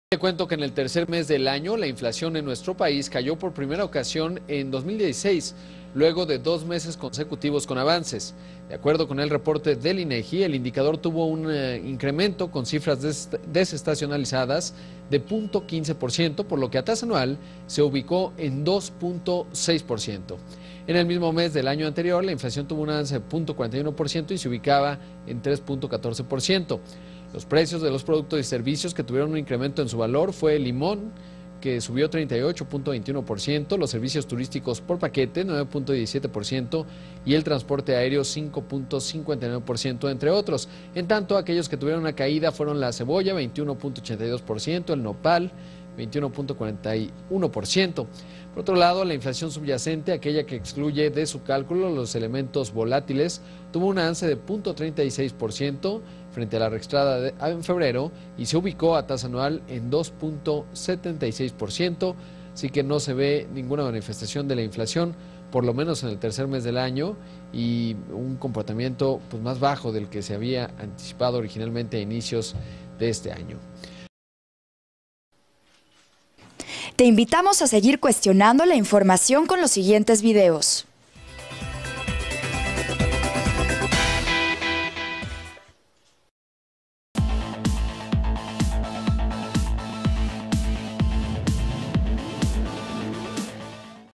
En marzo disminuyó la inflación en México por primera vez en 2016 / Rodrigo Pacheco
Durante el mes de marzo, la inflación en México, registró el primer descenso en 2016. Los meses anteriores se había incrementado din llegar al mínimo estimado.

07 de abril 2016